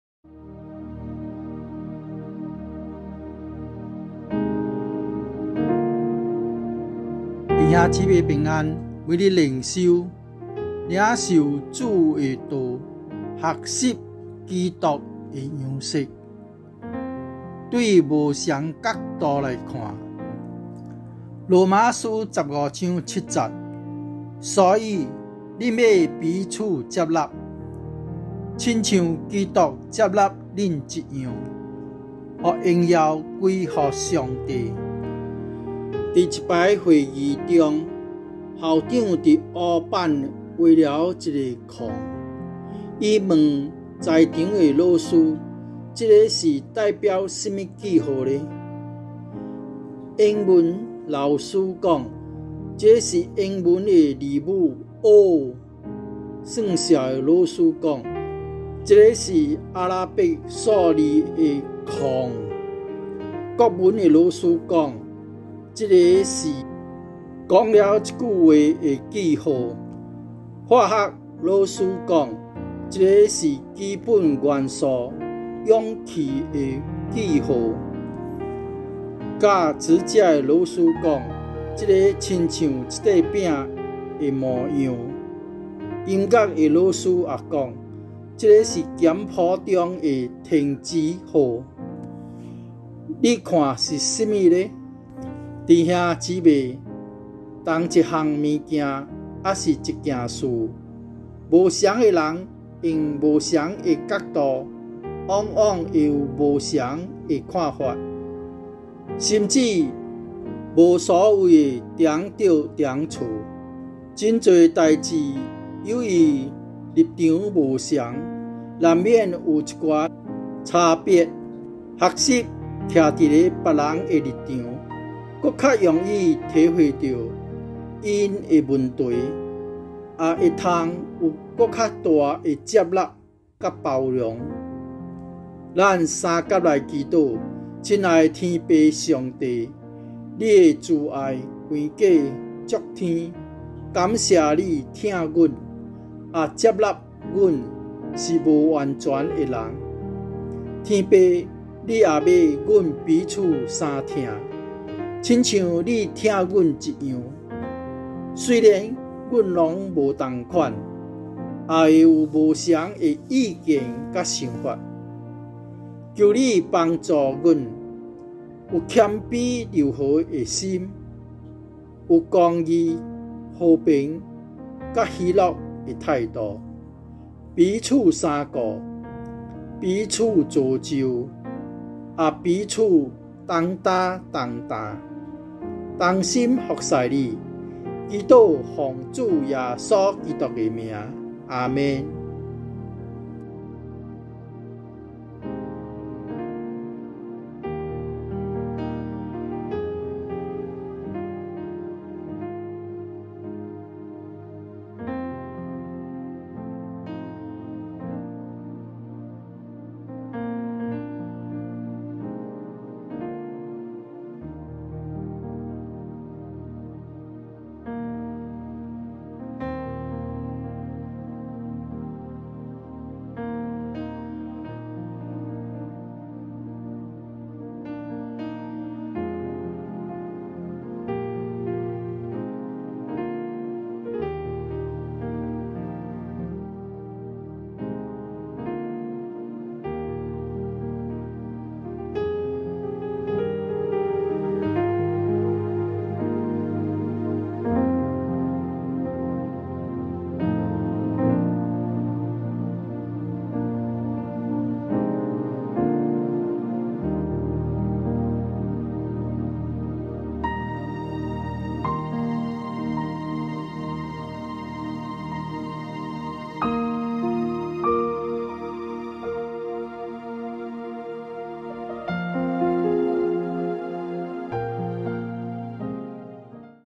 福建每日灵修 | 27-05-2023 | Hokkien Daily Devotion
领受主的道，学习基督的样式。
从不同角度来看

经文:
罗马书 15:7
所以，你们要彼此接纳，如同基督接纳你们一样，使荣耀归于神。

故事:
在一次会议中，校长在黑板画了一个“0”，他问在场的老师，这个是代表什么记号呢？
英文的老师说:「这个是英文的字母 0」
数学的老师说:「这个是阿拉伯数字的零。」
国文的老师说:「这个是说了一个话的句号。」
化学的老师说:「这个是基本元素氧气的符号。」
烹饪的老师说:「这个好像一个饼的模样。」
音乐老师也说:「这个是简谱中的停止符。」
你看是什么呢？

默想:
弟兄姐妹，同一个东西或是一件事，不同的人用不同的角度往往会有不同的看法，甚至无所谓的谁对谁错。许多事情由于立场不同，难免有一些差别。学习站在别人的立场，更加容易体会到他们的问题，也可以有更加大的接纳和包容。

祷告:
亲爱的天父上帝，祢的慈爱高过诸天。感谢祢爱我们，也接纳我们是不完全的人。天父祢也要我们彼此相爱，好像祢爱我们一样。虽然我们都不一样，也会有不同的意见和想法。求祢帮助我们，有谦卑柔和的心，有公义、和平与喜乐的态度。彼此相顾，彼此造就，也彼此担当重担，同心服事祢。祷告奉主耶稣基督的名，阿们。

(参考资料:小 故事大道理第67页)

©️ 版权归新加坡福建丰收所有